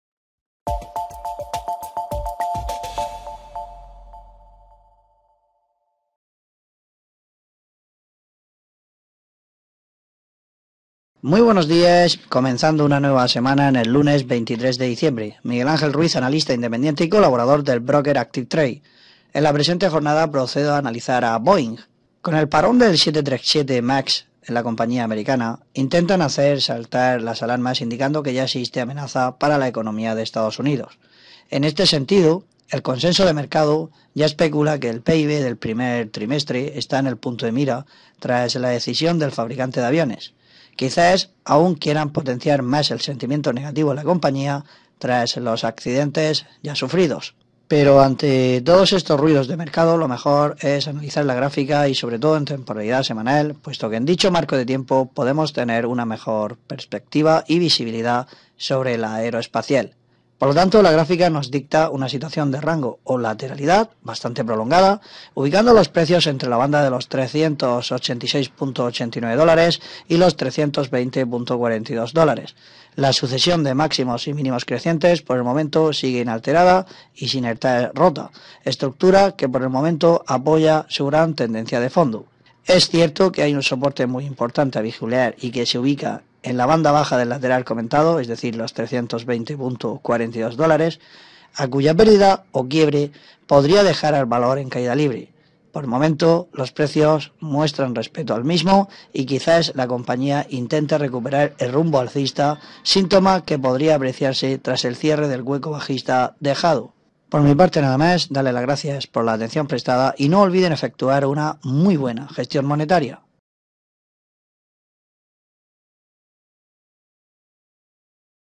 23/12/2019 - ¿Aterrizaje forzoso en Boeing?
✅ Pruebe las estrategias de negociación en una cuenta de demostración gratuita en tiempo real Sobre la americana están intentando saltar las alarmas sobre la posible amenaza para la economía de su respectivo país, tras el parón en la fabricación del 737 MAX. No obstante es importante la figura que la mercantil está dibujando en el análisis técnico y en temporalidad semanal, puesto que por el momento se sujeta a un importante soporte de relevancia. Para ver la cotización de Boeing en nuestra plataforma de trading ActivTrader, por favor clique